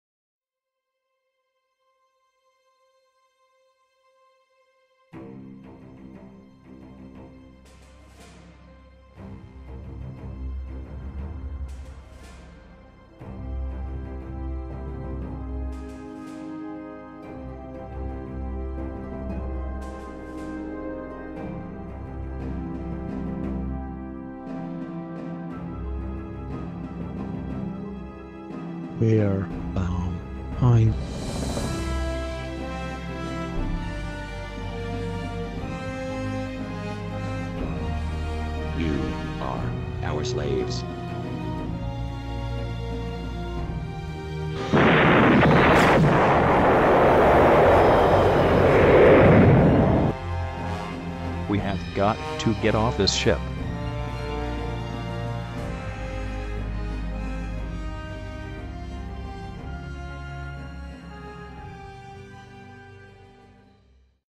WayFarer:Spaceship - Teaser Trailer {Extended}
Present day Earth. When a man is taken from his home, he finds himself a slave aboard an alien ship heading into the unknown.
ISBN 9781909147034